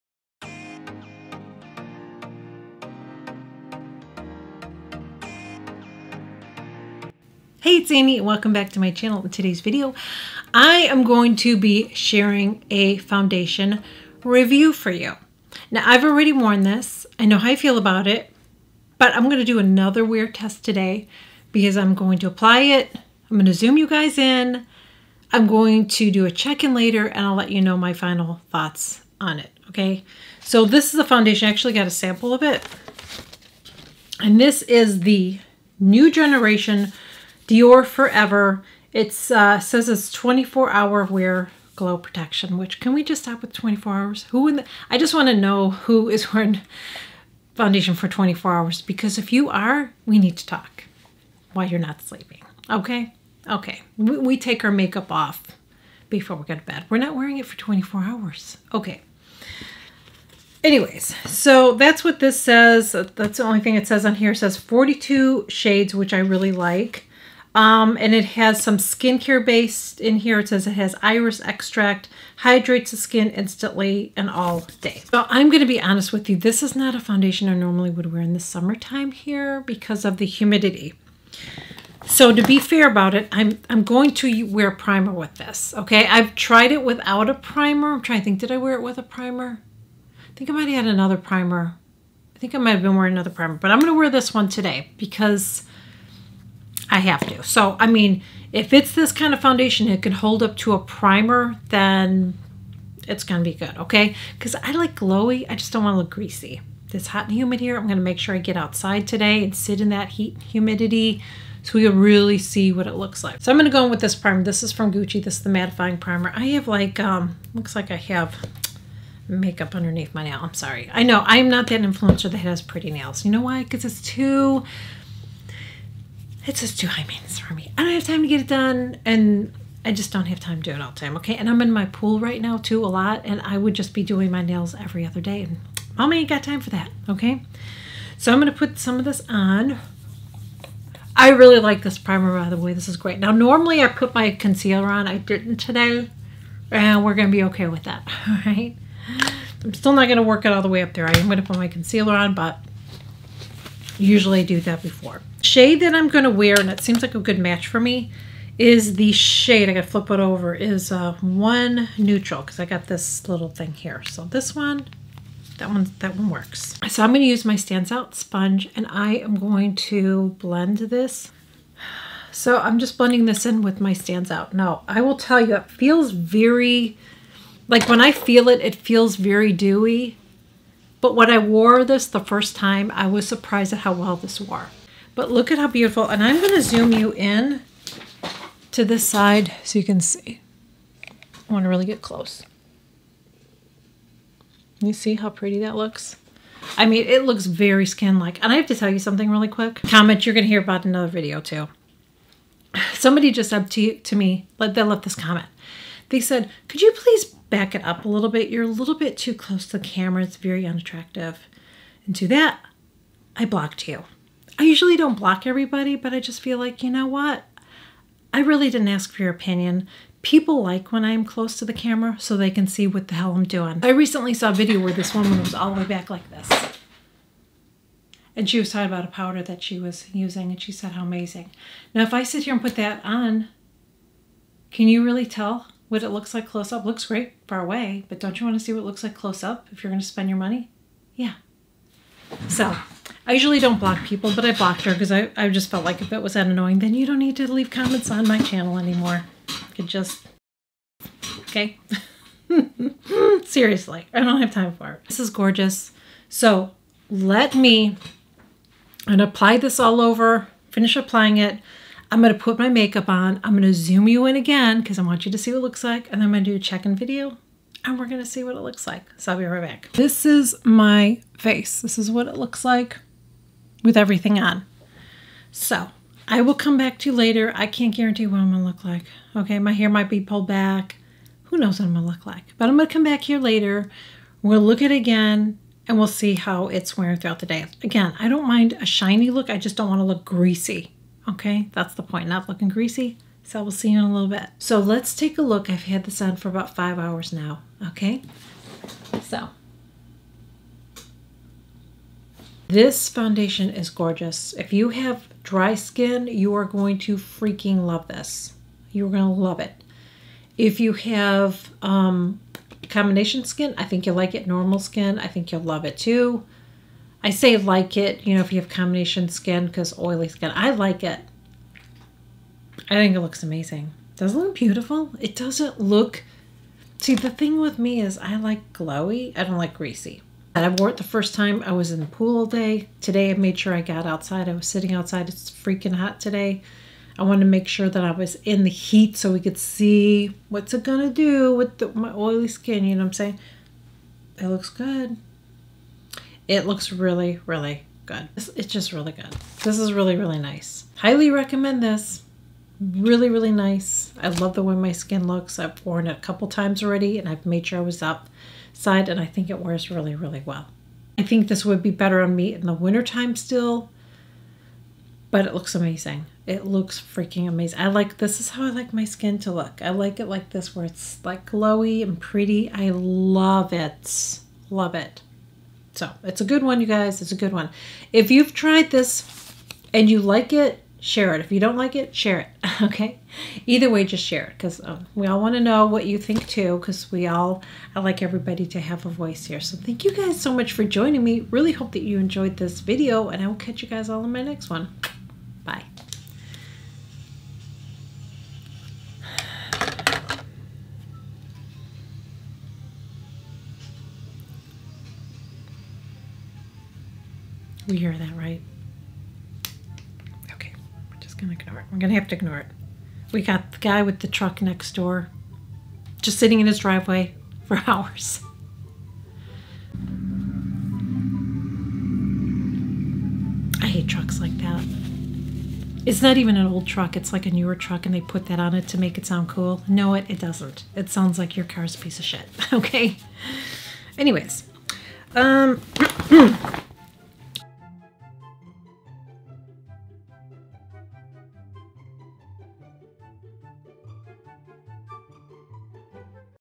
DIOR FOREVER SKIN GLOW FOUNDATION MATURE SKIN APPROVED?
Is the DIOR forever skin glow foundation mature skin approved? I'll be putting this to the test sharing my experience and thoughts wearing this in the heat and humidity in Florida.

MY READER GLASSES

WORN AND MENTIONED IN VIDEO-
DIOR FOREVER SKIN GLOW FOUNDATION-
DANESSA MYRICKS EYESHADOW TWIN FLAMES(MYSTIC FIRE)
LAURA MERCIER LIPSTICK (A LA ROSE) NLA